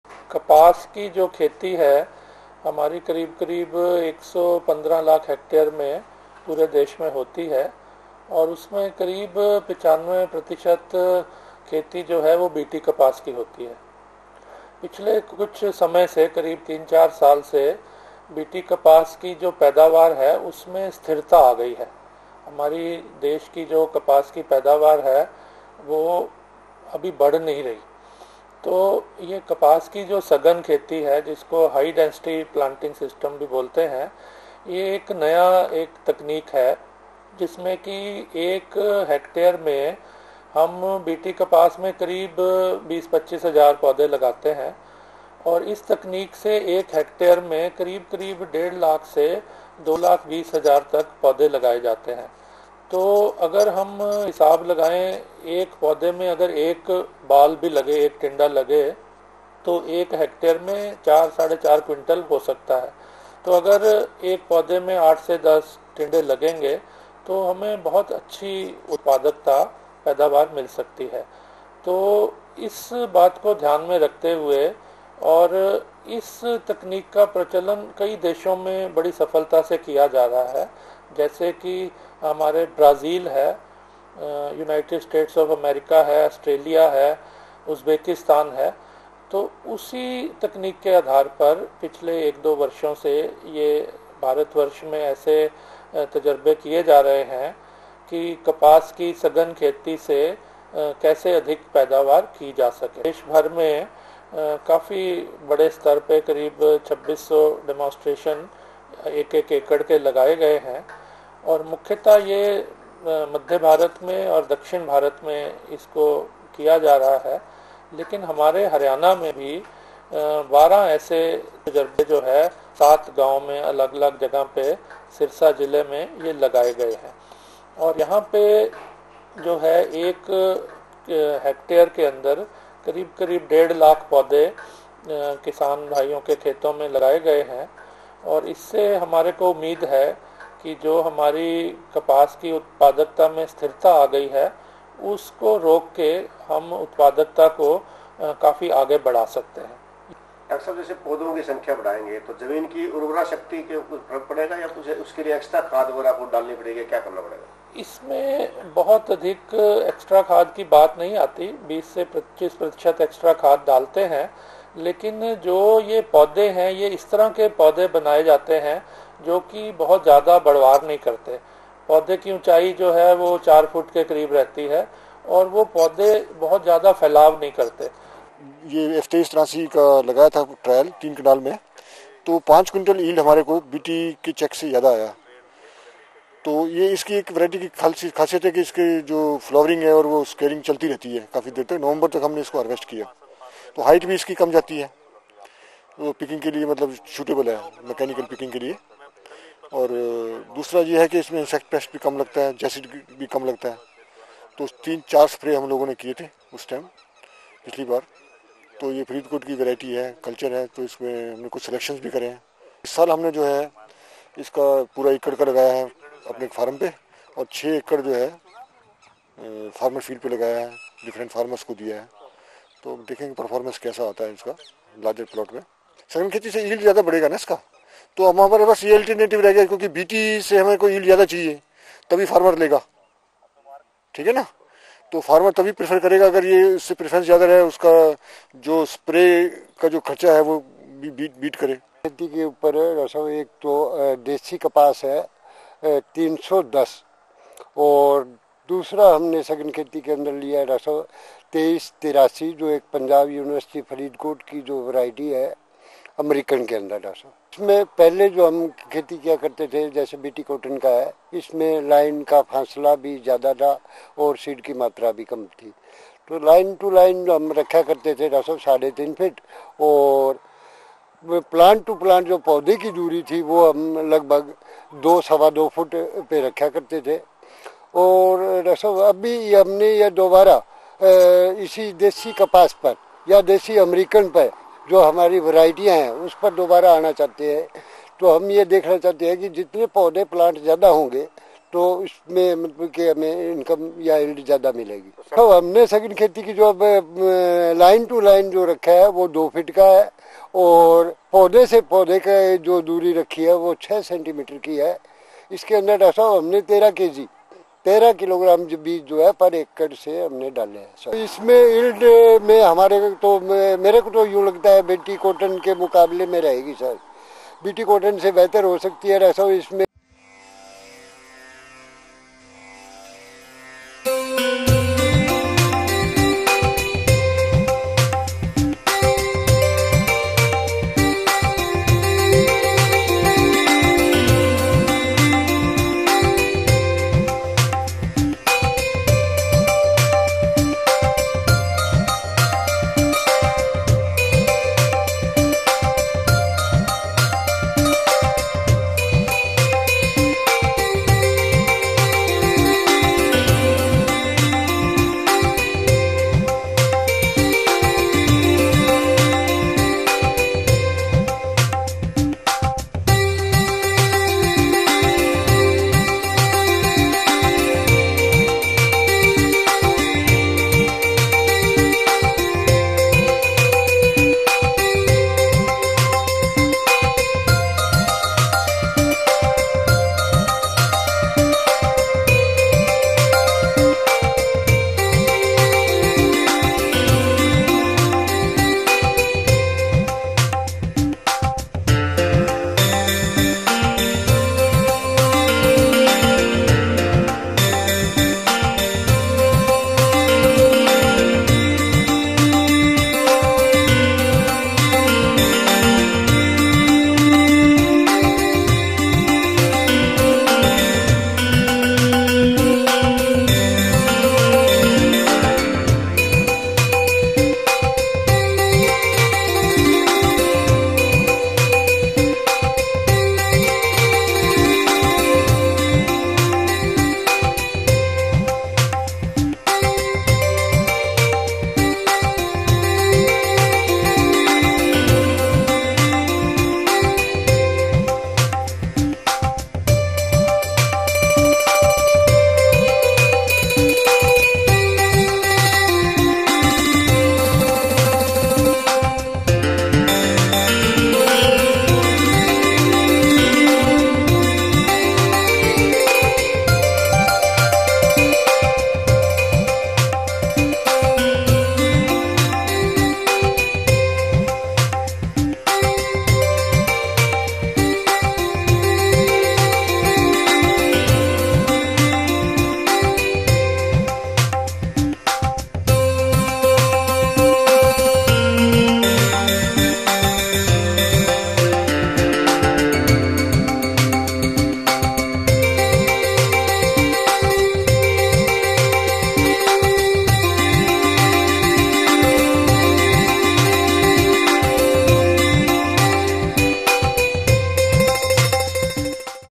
CICR Initiative High Density Cotton Sowing
CICR Initiative High: Density Cotton Sowing. Dr D Monga Station Head CICR Sira, Dr SL Ahuja project incharge, Dr Rishi, Farmer Raja Ram of Shahpur Begu speak about the project.